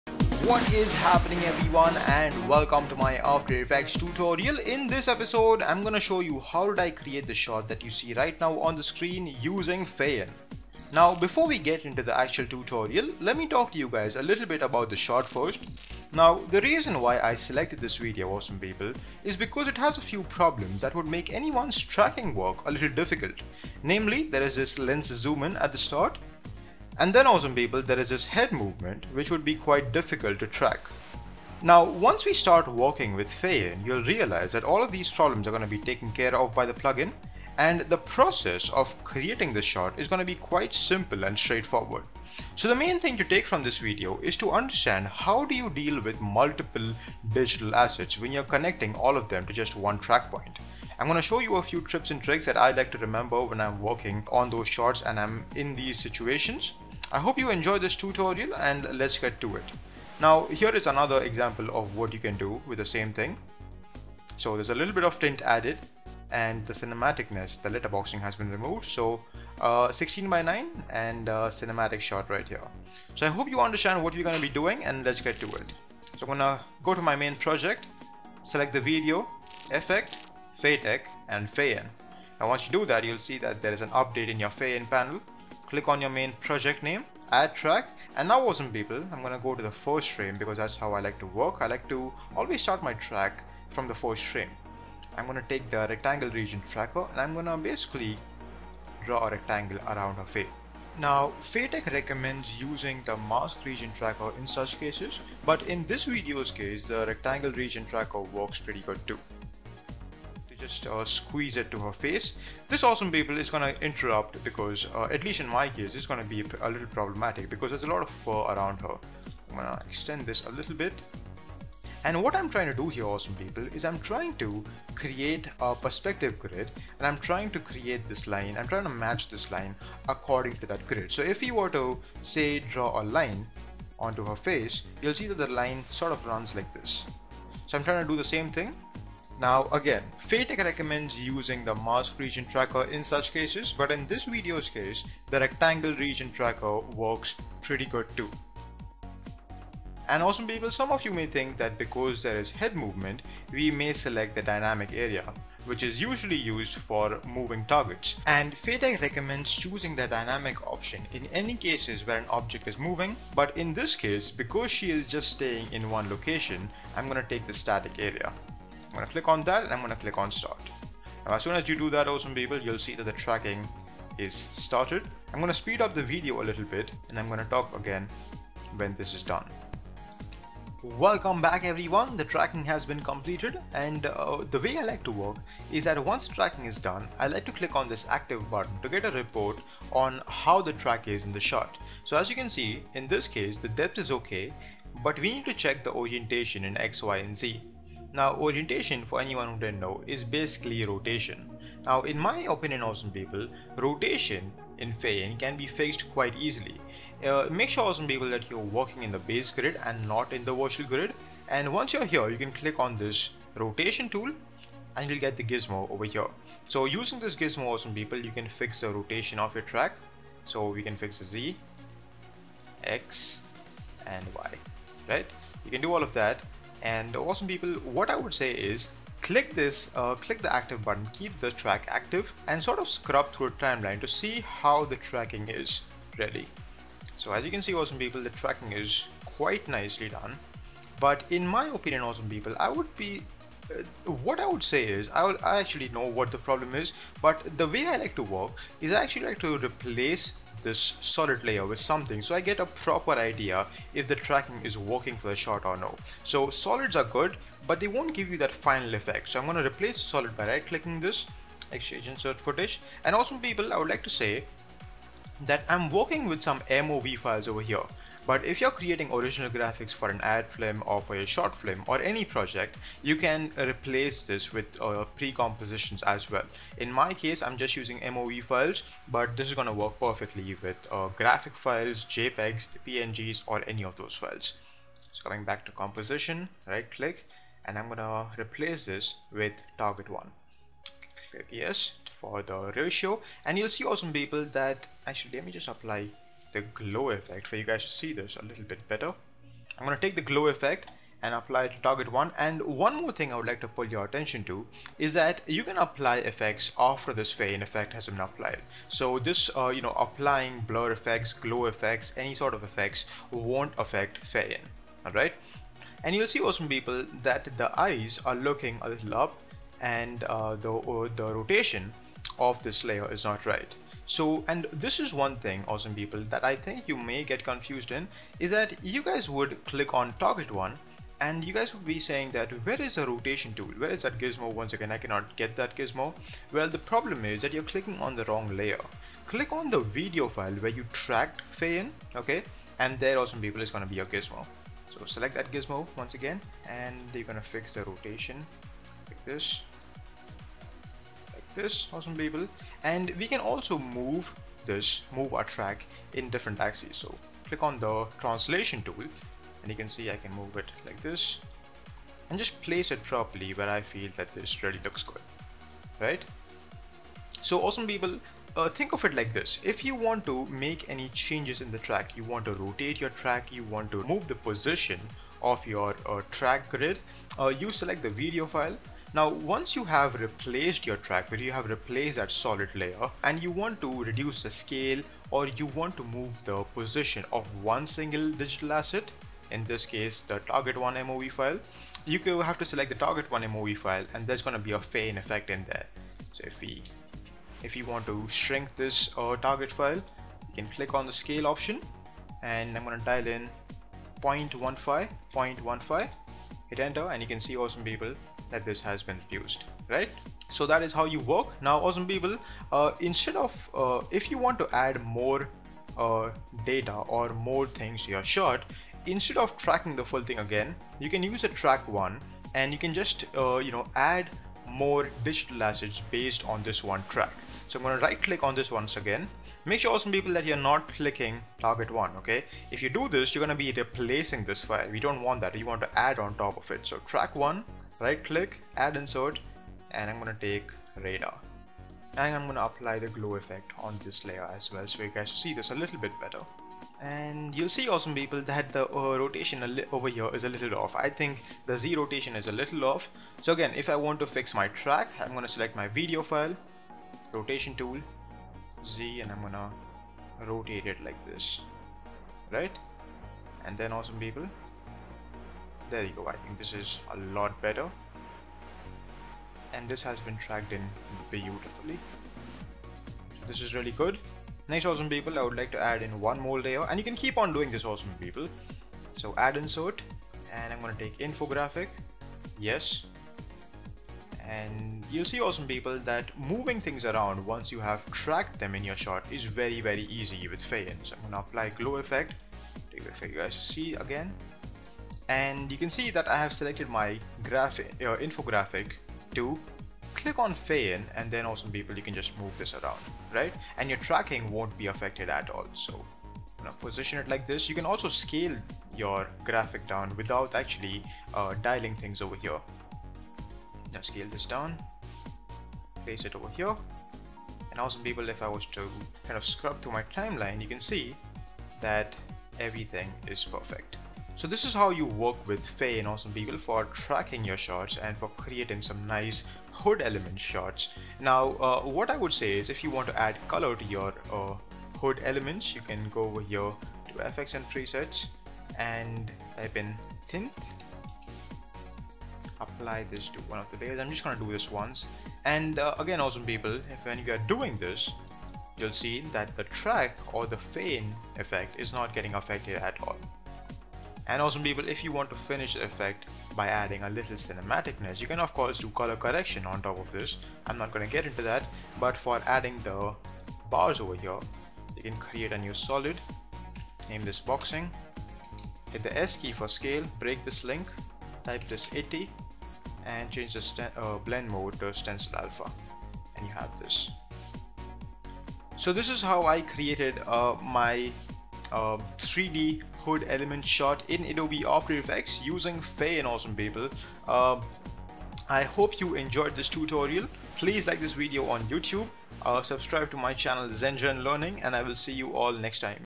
AE │fayIN Tracking Tutorials - 02 - Advanced 3D HUD Tracking!
TjStyle here and in this video I am gonna show you how to work with fayIN for creating a very popular HUD/Screen Interface Shot.

I am gonna show you how to work with Multiple Digital Assets and connecting them to one track point.

Get a 30 day trial of fayIN here -

Follow fayteq!

and

Premiere Pro